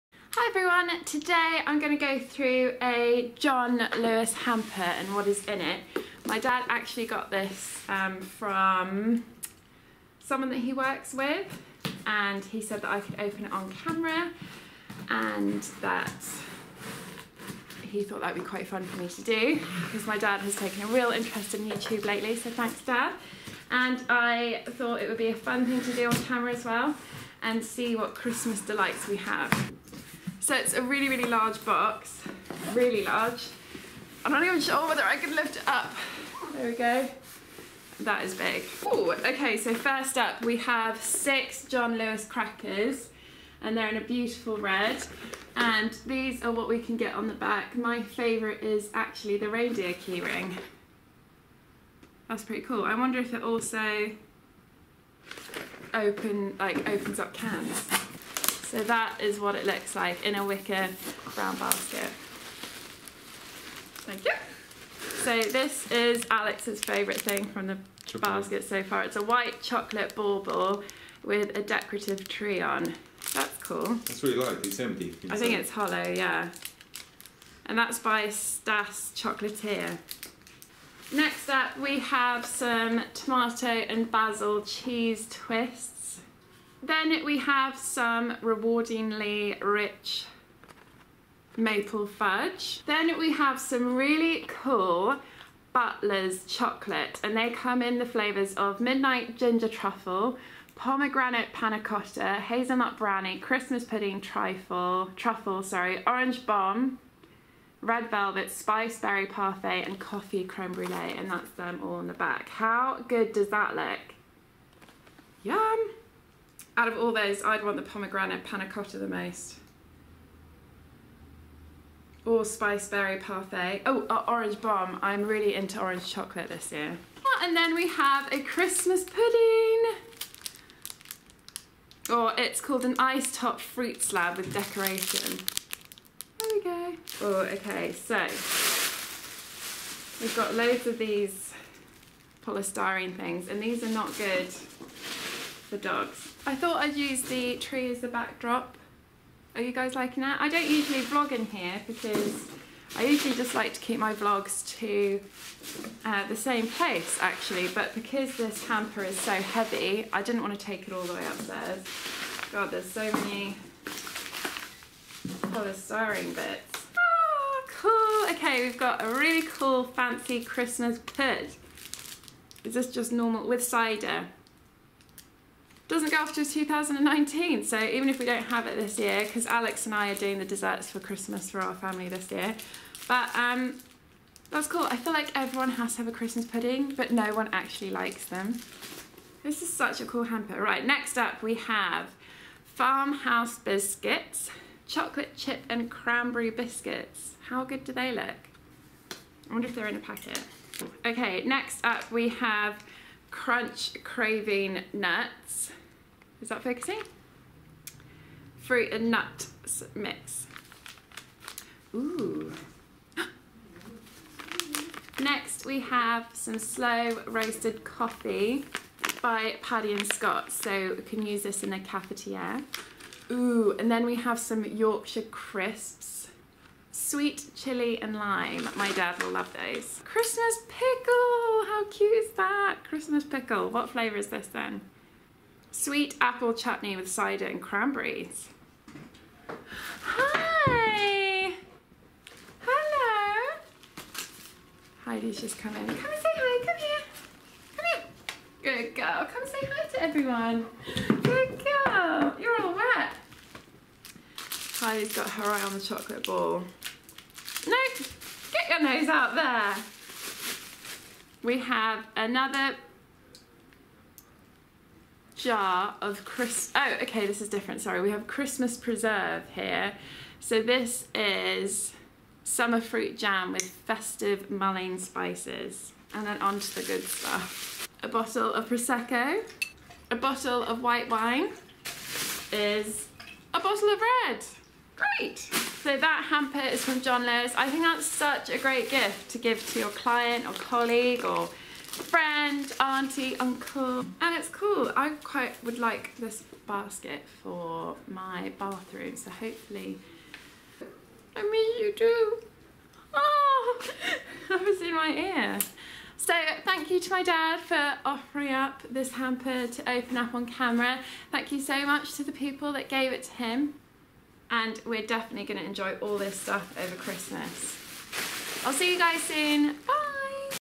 John Lewis Christmas Hamper | Gift Opening
My dad was given a John Lewis hamper and kindly let me open it on camera!

Hope you all had a great christmas and enjoy the festive season while you can! xx